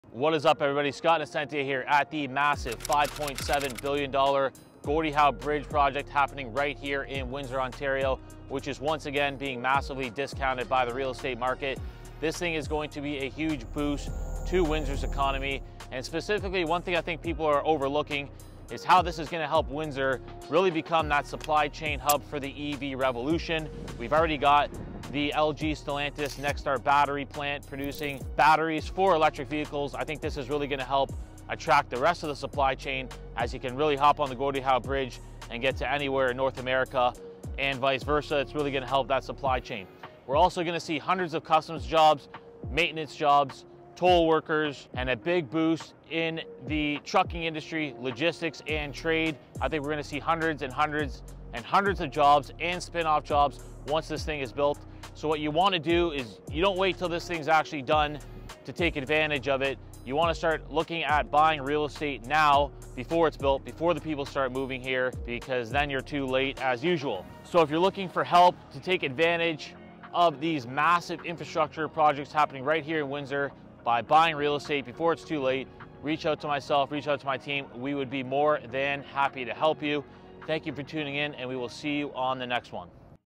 $5.7 Billion Gordie Howe Bridge
Construction of the $5.7 billion Gordie Howe Bridge is underway and it is poised to have a major impact on the Windsor real estate market.

Drop a comment for any questions you have about real estate and we'll make a video to specifically answer your question